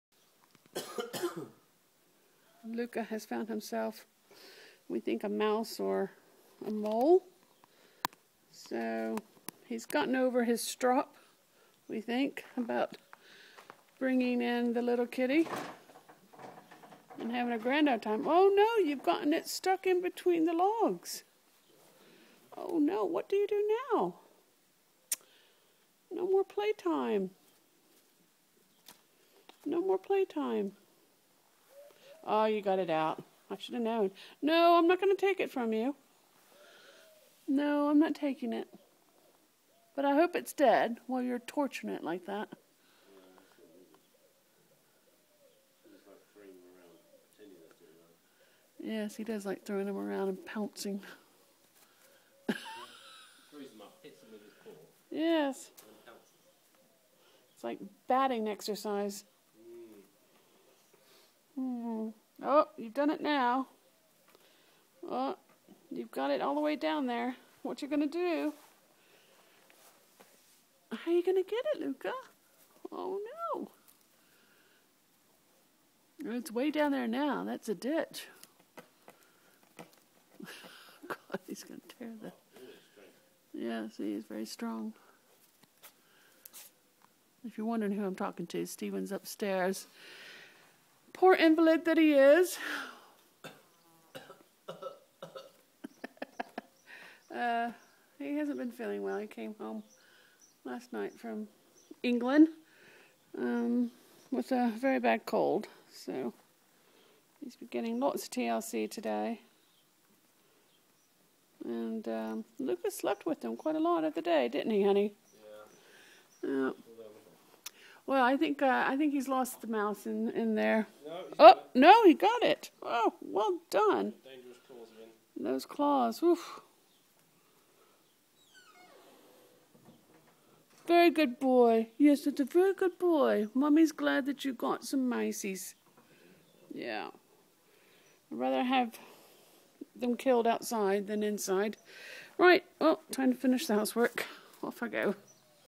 Lucca Hunting Bats, Mice and Moles
1 July 2011. Year 1, Week 21. Seems Lucca has found his batsman, or rather batsmouse. Having escaped Sienna, Lucca runs about playing outside.